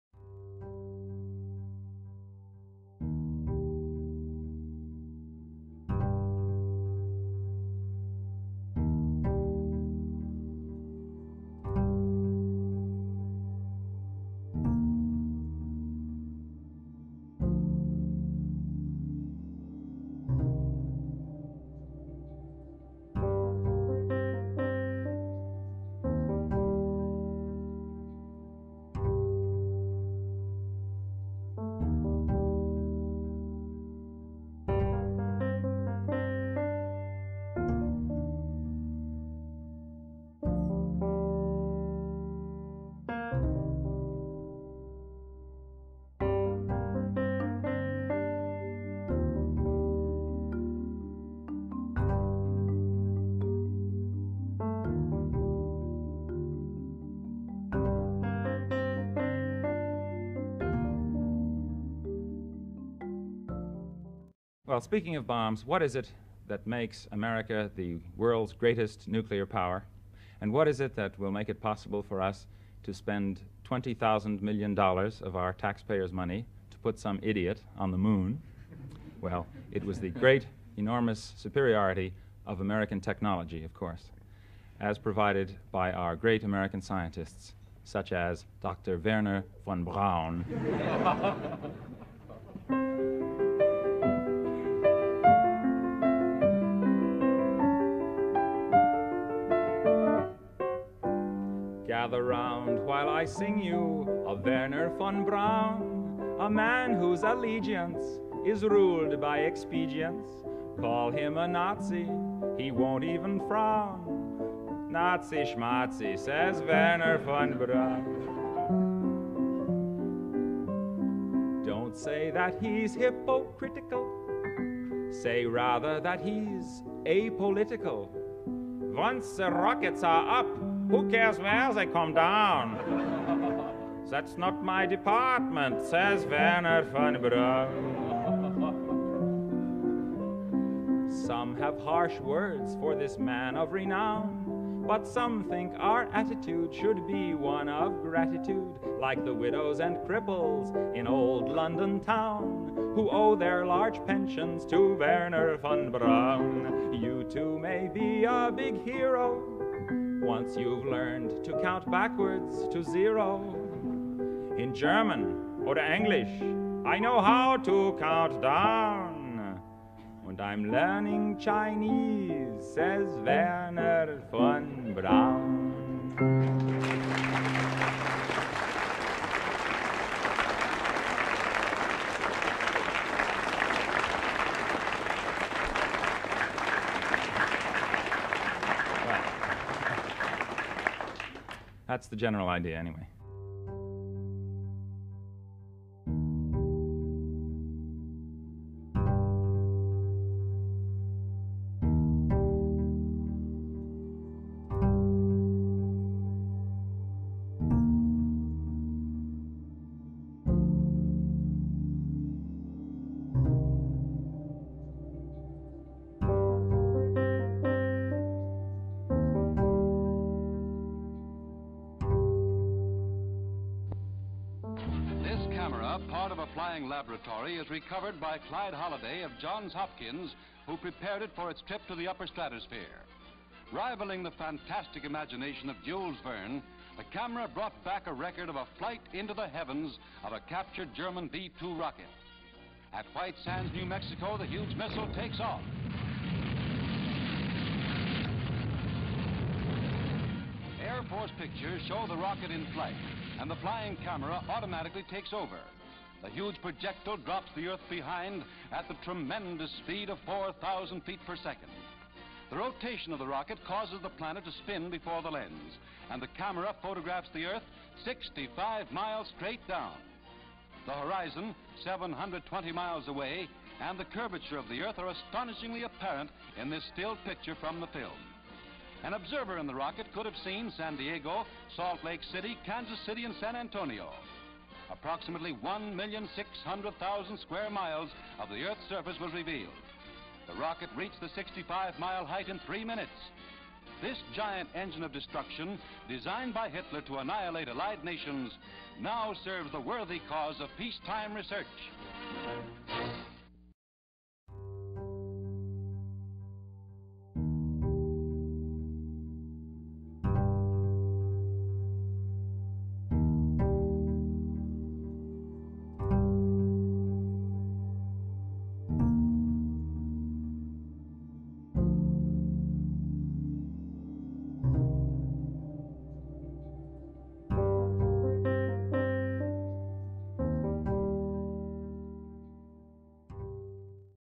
Werhner Von Braun, missile V2 et Terre plate
Vidéo sur Werhner Von Braun, ingénieur nazi devenu américain grâce à l'opération paperclip
Il est de la nasa dès sa création en 1958
Il conçoit le missile V2, qui a à son bord en 1946, une caméra qui nous donne le premier cliché de la Terre depuis une altitude de 105 Km